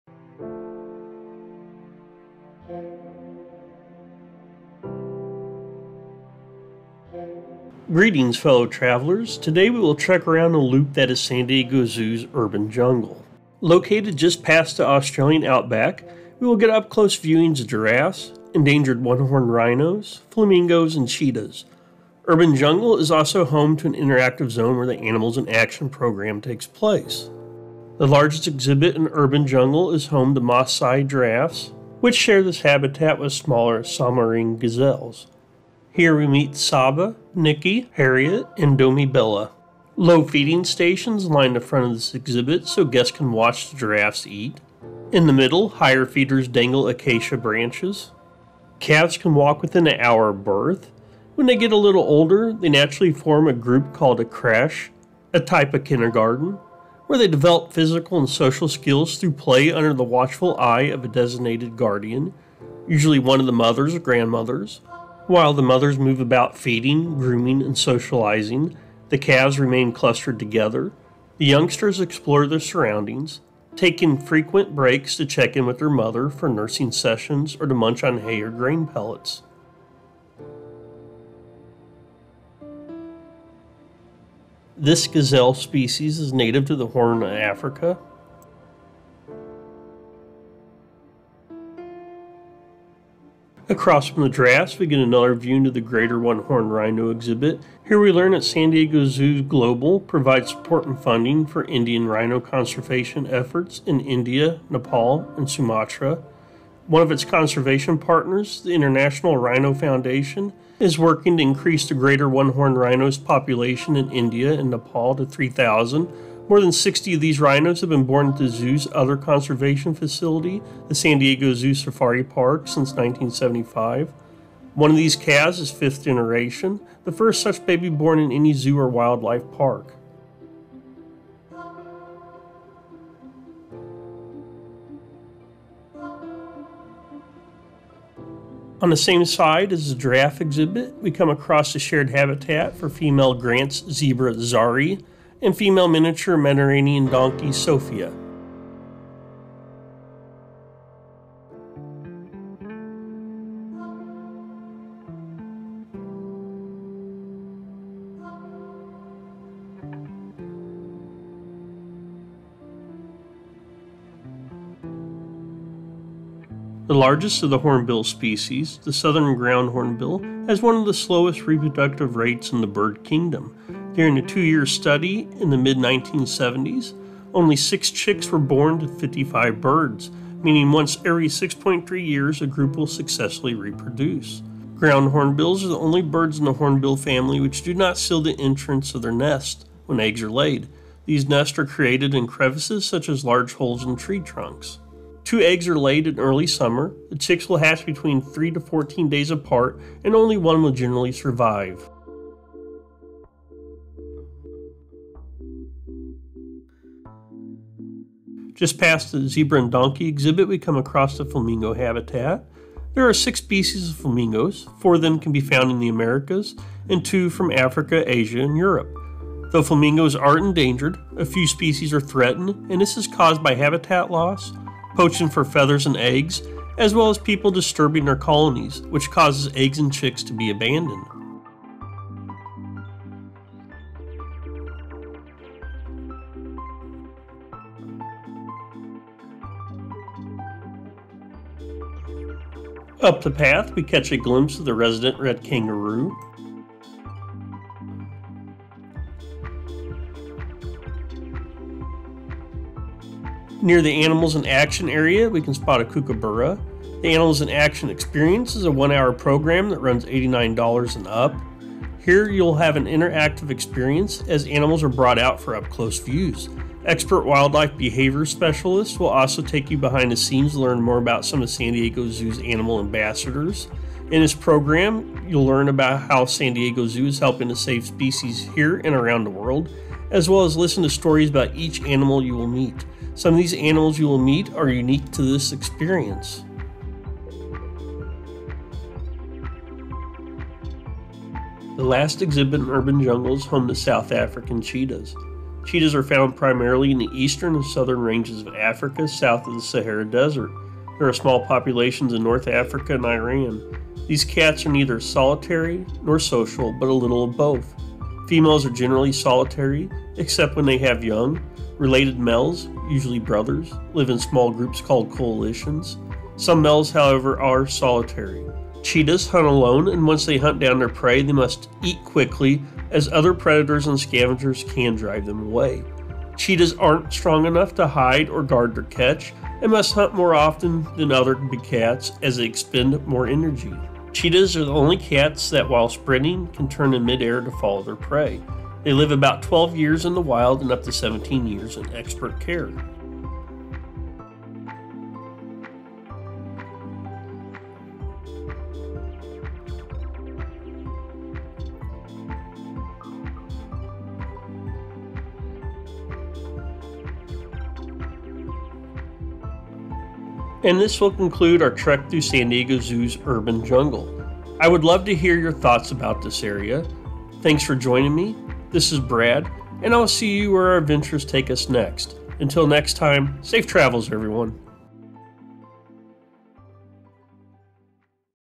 Urban Jungle | San Diego Zoo
San Diego Zoo  Urban Jungle

Located near the Australian Outback we find the Urban Jungle. Here we will get up close viewings of Giraffe's Endangered one-horned rhinos, Flamingos and Cheetahs. San Diego Zoos Urban Jungle is also home to an interactive zone where the Animals in Action program takes place.

The largest exhibit in Urban Jungle is home to Masai Giraffes, which share this habitat with the smaller Soemmerring's Gazelle. Low feeding stations line the front of the exhibit so guest's can watch the giraffe's eat.

The middle area is home to the Greater One-Horned Rhino (Indian Rhino) here we learn that San Diego Global provides support and funding for Indian Rhino conservation efforts in India, Nepal and Sumatra.

On the other side of the Giraffe habitat we find a shared habitat for Female Grant's Zebra Zari & Female Miniature Mediterranean Donkey Sophia.

Other exhibits include Southern Ground Hornbills, Flamingos, Red Kangaroo' and Fennec Fox.

Near Animals in Action area we can see a Kookaburra. The Animals in Action Experience is a one hour program that has an extra charge, here you will have an interactive experience as animals are brought our for up-close iews. Expert wildlife behavior specialists will also take you behind the scenes to learn more about the Animal Ambassadors.

The last exhibit in Urban Jungle is home to South African Cheetah's.

Species List (Displayed in March of 2021)
Masai  Giraffe
Soemmerring's Gazelle
Greater One-Horned Rhino
Grant's Zebra
Miniature Mediterranean Donkey
Southern Ground Hornbill
Flamingos
Red Kangaroo
Kookaburra
Fennec Fox
South African Cheetah

Thanks for joining me on this trek through the Urban Jungle at San Diego Zoo!                                                                         -Brad